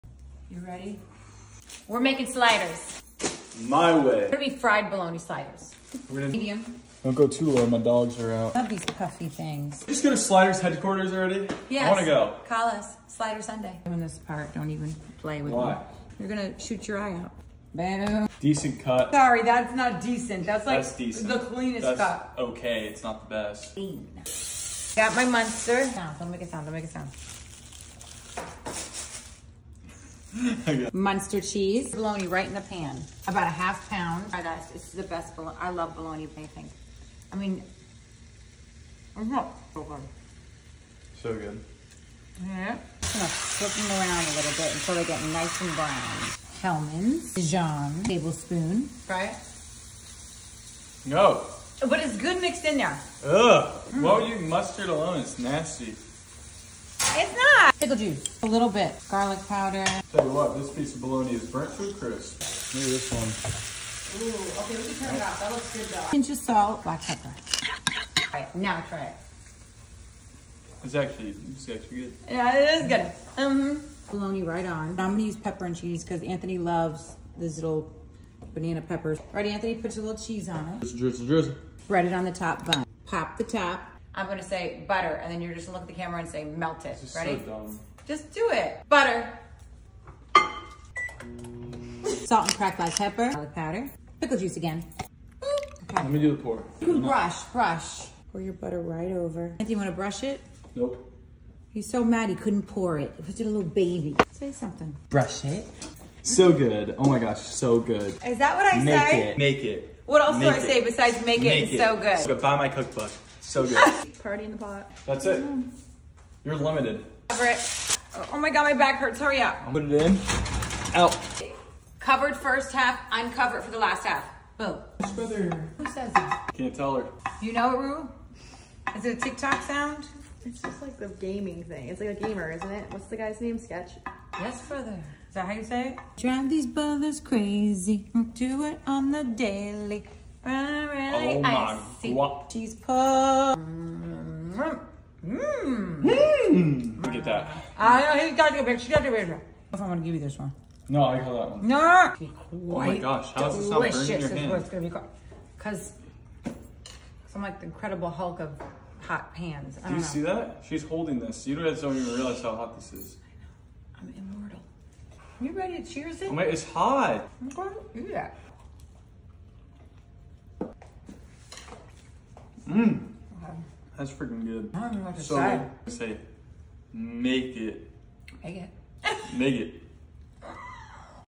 Mother Son Cooking Fried Bologna Sliders
@melissajorealrecipes and Anthony (my son) cook his favorite sliders! Fried bologna sliders! Let us know if you want more long form content! This recipe is soooo good! You have to try it!
Ingredients:
1 pack original Kings Hawaiian rolls
1/2 lb deli bologna, sliced thin
4 slices of muenster cheese
3 oz freshly shredded mozzarella
2 tbsp pepperoncinis (or more if you want)
2 tbsp cooking oil
3 tbsp mayo
1 heaping tbsp dijon mustard
2 tsp pickle juice, divided
1/2 tsp + 1/4 tsp garlic powder, divided
1/2 tsp + 1/4 tsp black pepper, divided
Pinch of salt
1/2 stick butter, melted
Directions:
Slice the Hawaiian rolls open and spray a baking dish with cooking spray. Place the bottom buns in the baking dish and add on the slices of muenster cheese.
In a pan with oil add your slices of bologna and toss it around on medium-high heat until they're browned and slightly crispy.
Once the bologna is done place it on top of the muenster cheese.
Add on the pepperoncinis and sprinkle over the shredded mozzarella cheese.
To make the sauce combine mayo, dijon, 1 tsp pickle juice, 1/2 tsp garlic powder, 1/2 tsp black pepper, and a pinch of salt. Evenly spread this onto the top buns.
Then top the sliders with the top buns.
Combine the melted butter, 1/4 tsp garlic powder, 1 tsp pickle juice, and 1/4 tsp  black pepper. Evenly pour this butter mixture over the top of the sliders.
Bake these at 350°F covered for 25 minutes. Then uncover it and bake for an additional 5 minutes. Enjoy!!